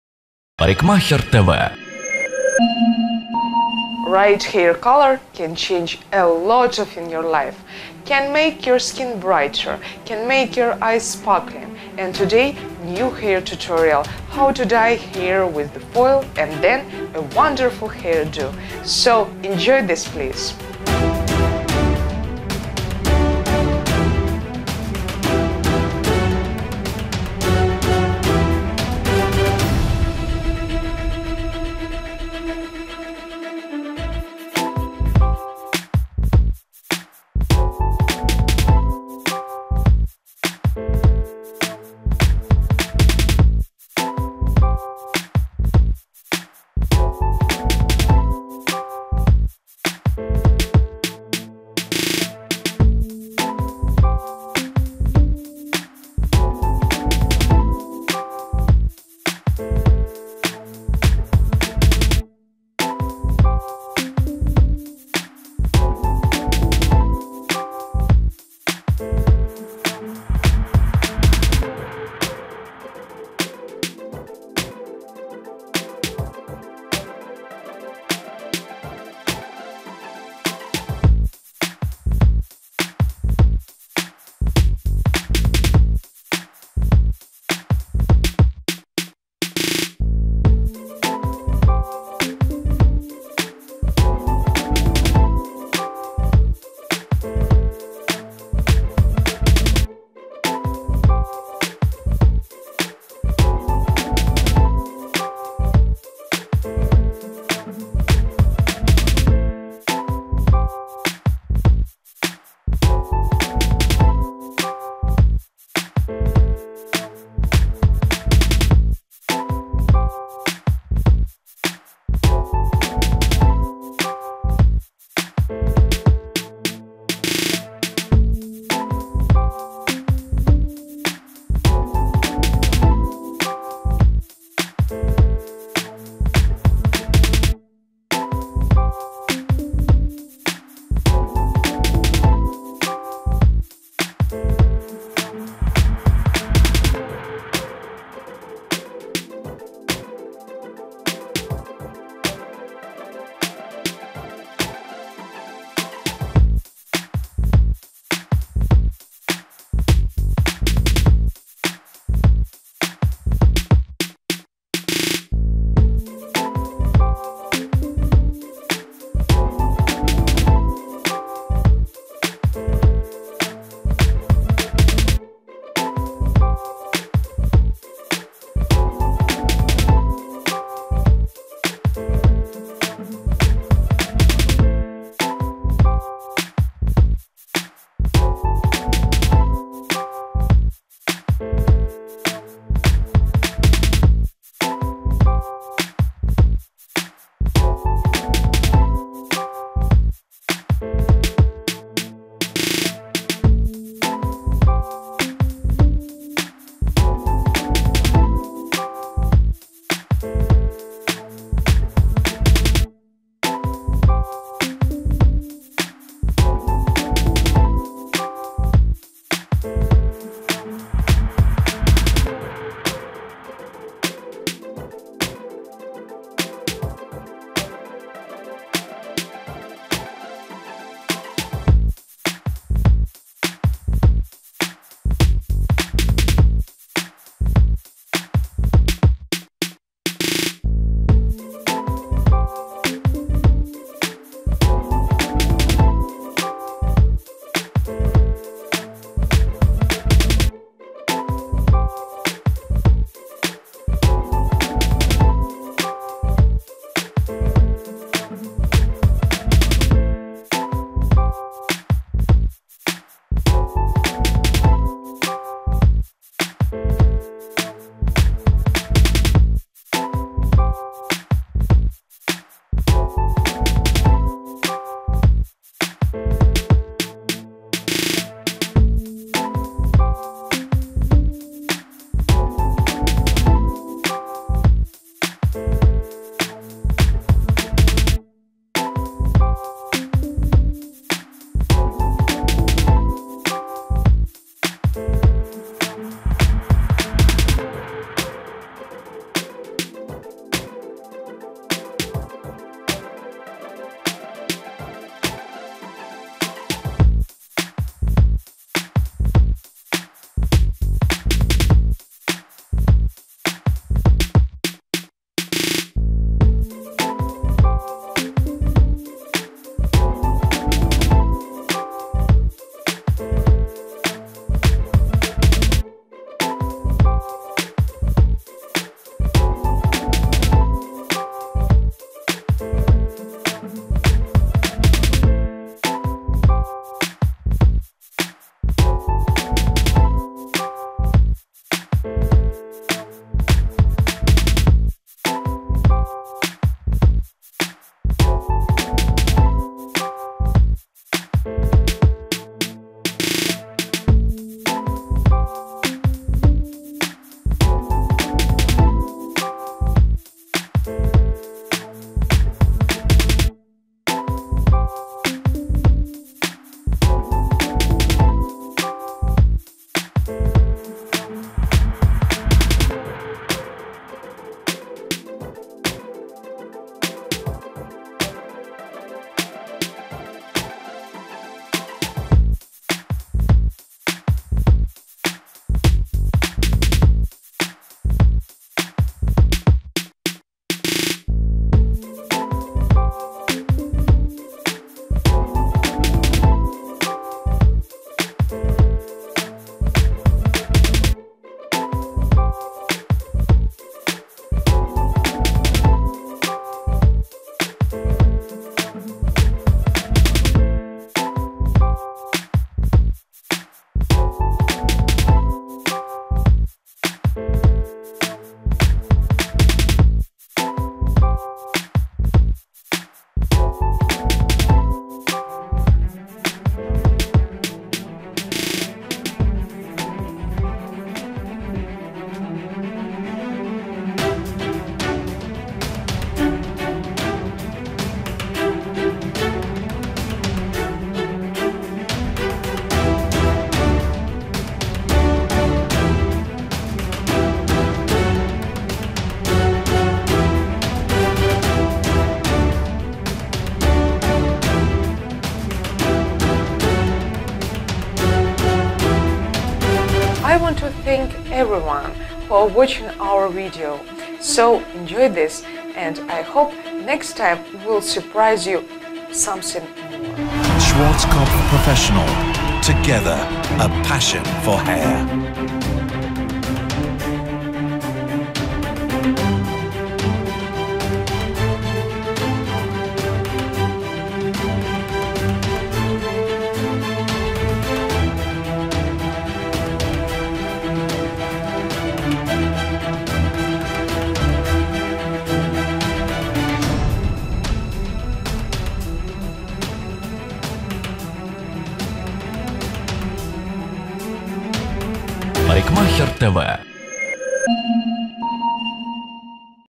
How to dye your hair + short haircut. Sergey Zima parikmaxer tv english version
Looking for hair color ideas? Feeling a bit blue and want to update your look for next season? Well, you should try a new hair color! We have just the thing for you.